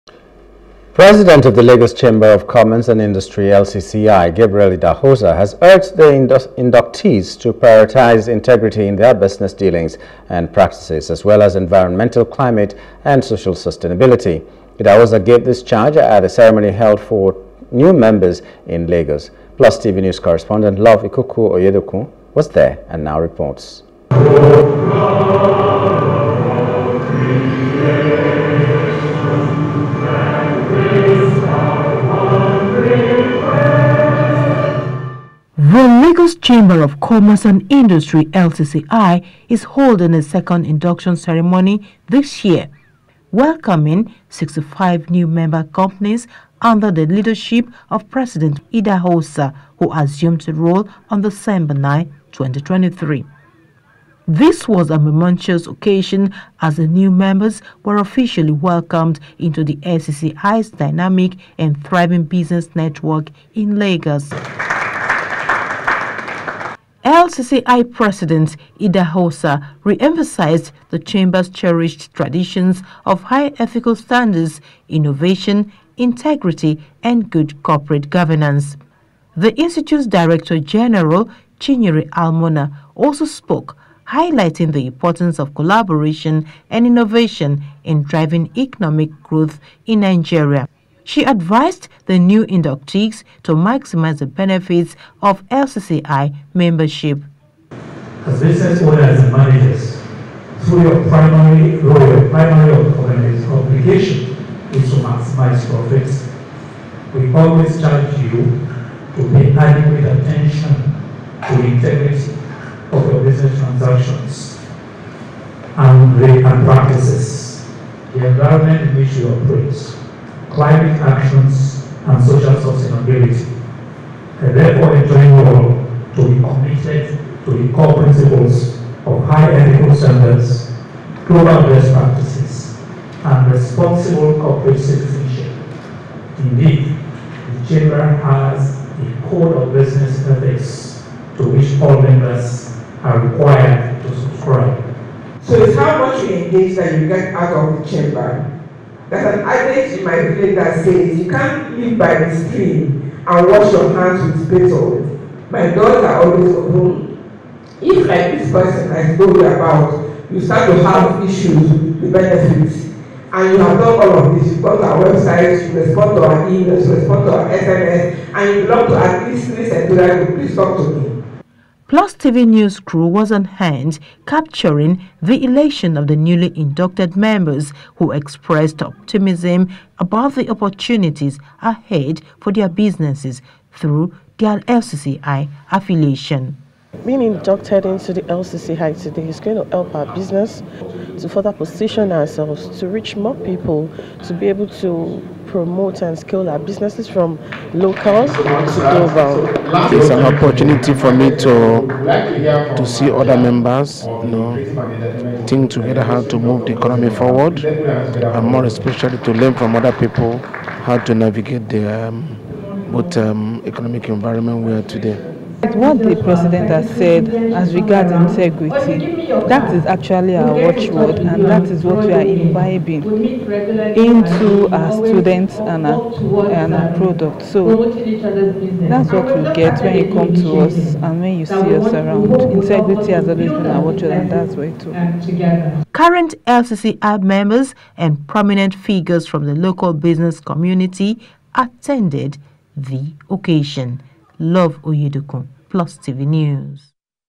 LCCI: Induction New Members Charged On Integrity In Business Practices New
President of the Lagos Chamber of Commerce and Industry (LCCI), Gabriel Idahosa has urged the inductees to prioritize integrity in their business dealings and practices, as well as environmental, climate, and social sustainability. Idahosa gave this charge at a ceremony held for new members in Lagos. Plus, TV News correspondent Love Ikuku-Oyedokun was there and is now reporting.